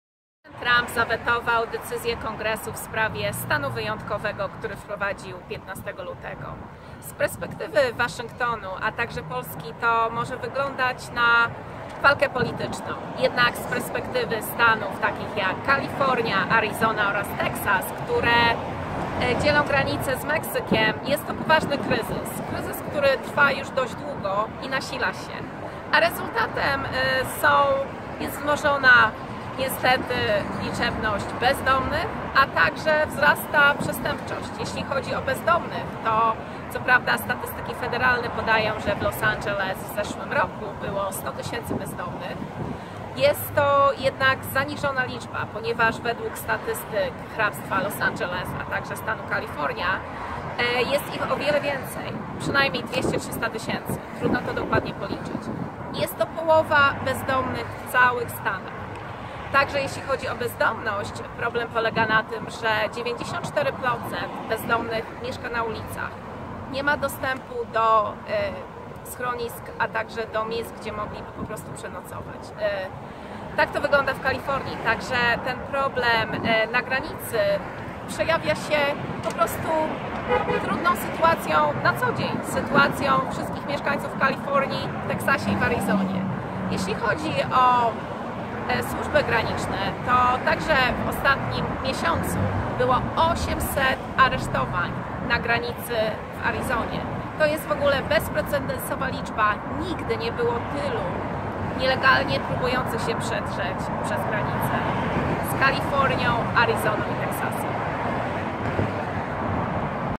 NASZA RELACJA Z LOS ANGELES / Co weto Trumpa oznacza dla Kalifornii? Jakie będą skutki?
Prezydent Donald Trump zawetował wczoraj decyzję Kongresu USA ws. stanu wyjątkowego, który wprowadził, aby uzyskać fundusze na budowę muru na granicy z Meksykiem. Co to oznacza? Specjalnie dla Niezależna.pl korespondencję z Los Angeles przekazała Małgorzata Schulz.
Trump ogłosił pierwsze weto swej prezydentury nazajutrz po przegłosowaniu w Senacie decyzji o odwołaniu rozporządzenia prezydenta. Taką samą rezolucję Izba Reprezentantów przyjęła 26 lutego. Kongres uznał, że prezydent przekroczył swe uprawnienia, usiłując zdobyć dzięki ogłoszeniu stanu wyjątkowego fundusze, których zdecydowanie odmówili mu kongresmeni.

W tej sytuacji Izba Reprezentantów podejmie - jak zapowiedziała jej przewodnicząca Nancy Pelosi - próbę oddalenia weta w kolejnym głosowaniu, które nastąpi 26 marca. Trump ma sporą szansę wygrać konfrontację z Kongresem, ponieważ aby można było oddalić prezydenckie weto w obu izbach, musi za tym zagłosować dwie trzecie kongresmenów, a szanse na to są niewielkie, choć Demokraci mają większość w Izbie Reprezentantów.